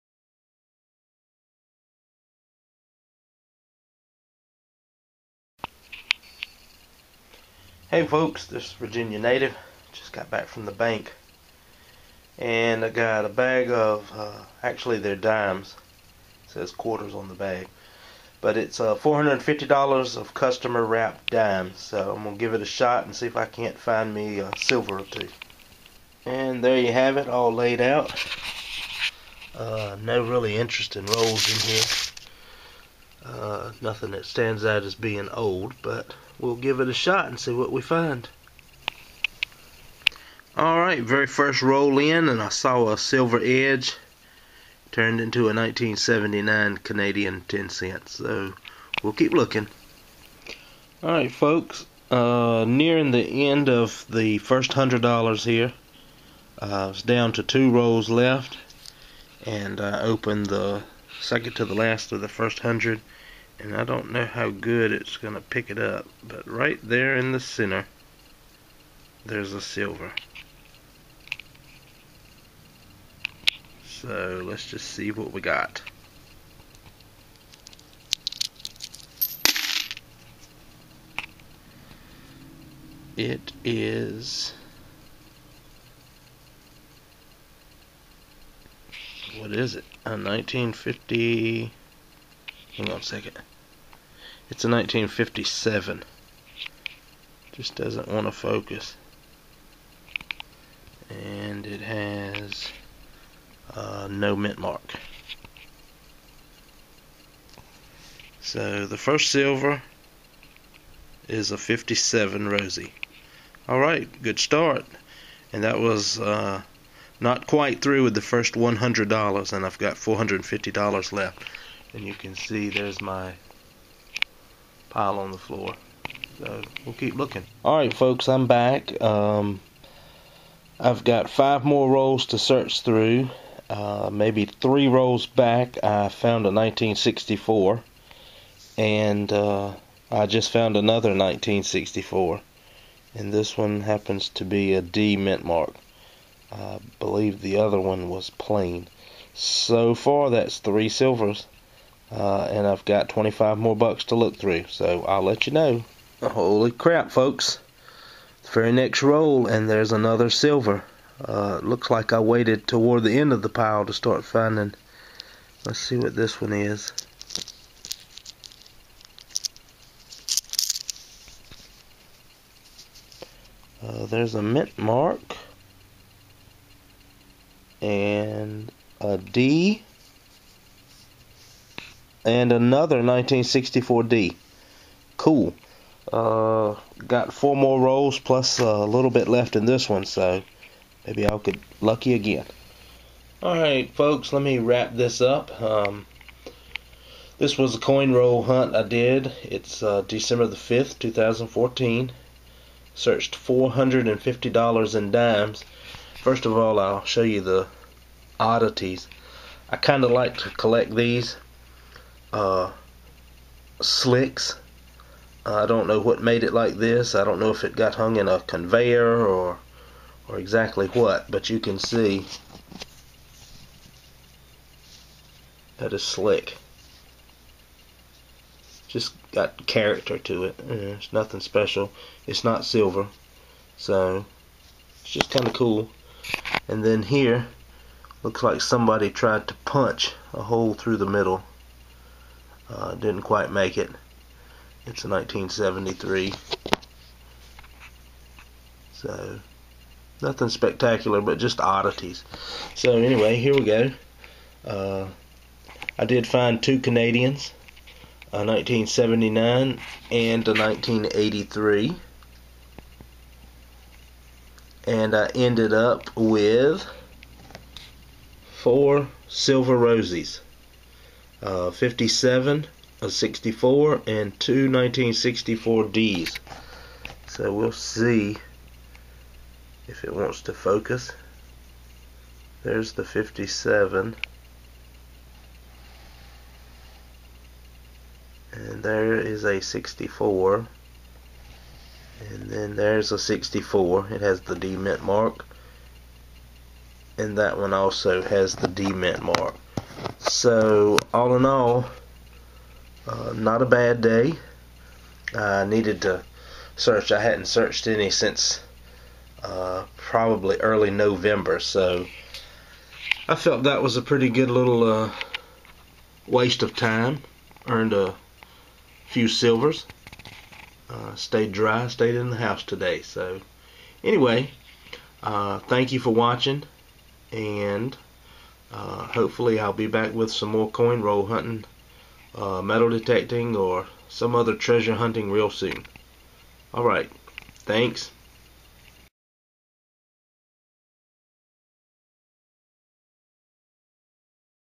Coin Roll Hunting - December 5, 2014
I searched $450 in dimes today and here are the goodies I found!